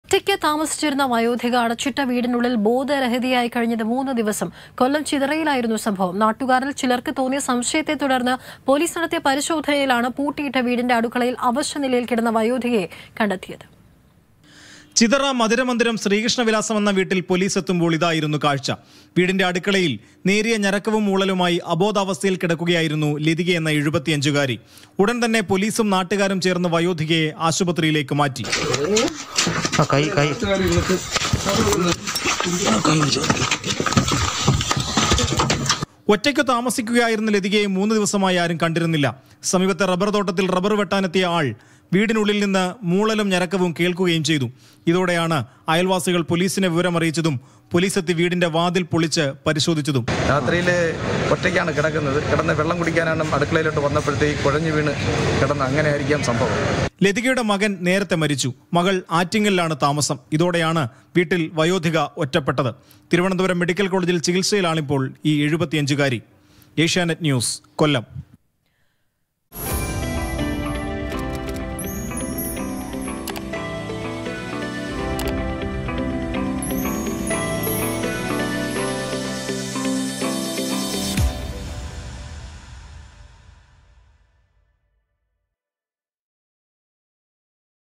അടച്ചിട്ട വീടിനുള്ളിൽ ബോധരഹിതയായി മൂന്ന് ദിവസം; വയോധികയുടെ ദുരിതം | Old Woman Life | Kollam
ഒറ്റയ്ക്ക് താമസം, അടച്ചിട്ട വീടിനുള്ളിൽ ബോധരഹിതയായി കഴിഞ്ഞത് മൂന്ന് ദിവസം. കൊല്ലത്തെ വയോധികയുടെ ദുരിതം.
Pathetic life of Old woman in Kollam.